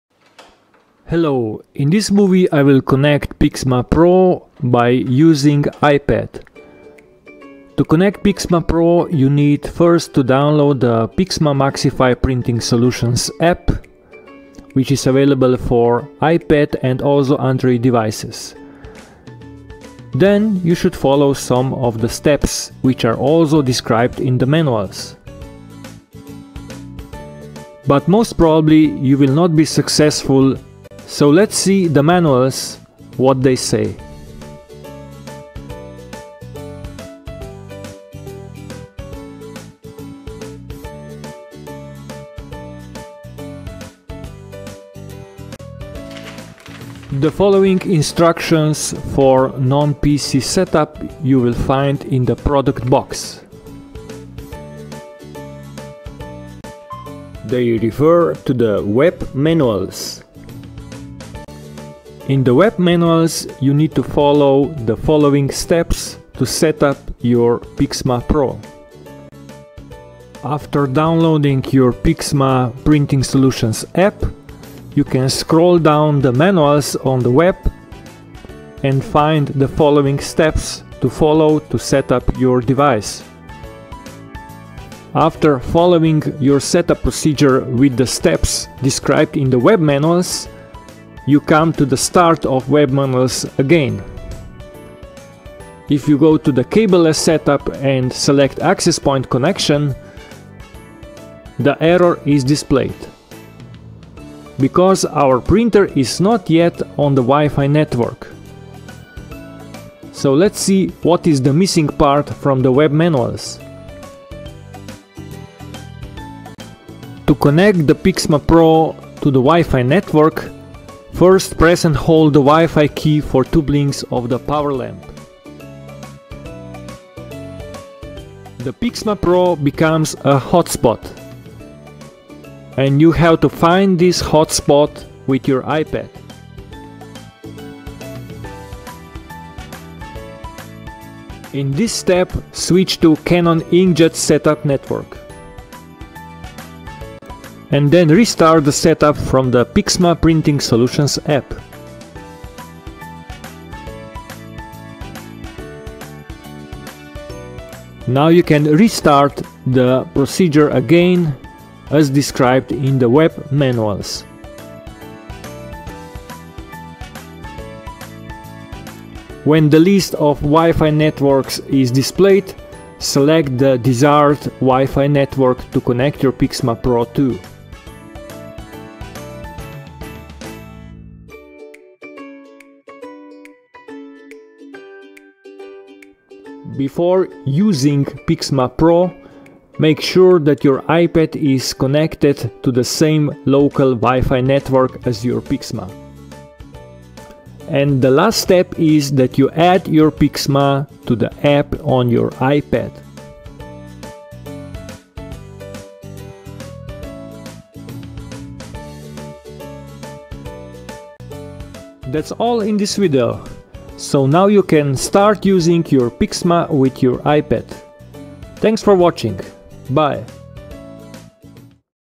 Pixma PRO-100S Setup using iPad
In this movie I show how to setup the Pixma PRO to wifi network using iPad. At 2:00 you can find the main hint for setting up, because you need to put Pixma to hotspot mode for setting up.